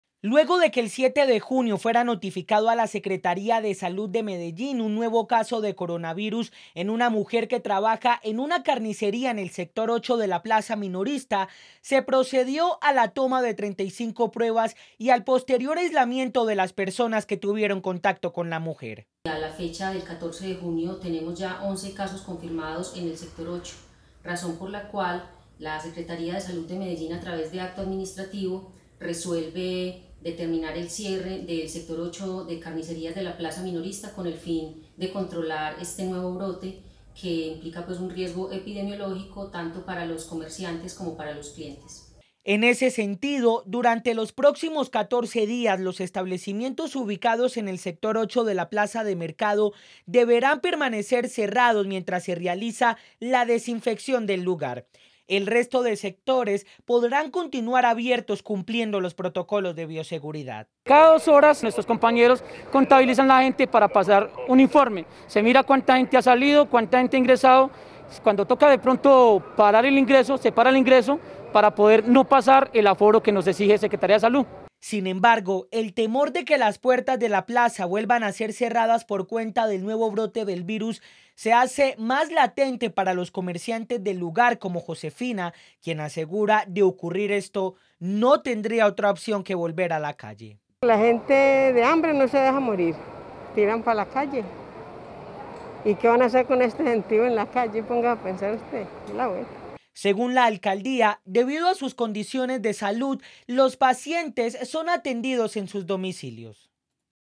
Por 11 casos nuevos se ordenó el cierre parcial de la Plaza Minorista - Telemedellín
La Plaza Minorista de Medellín vuelve a ser cerrada, esta vez de manera parcial por un periodo de 14 días, tras confirmarse un nuevo brote de coronavirus en el interior del lugar.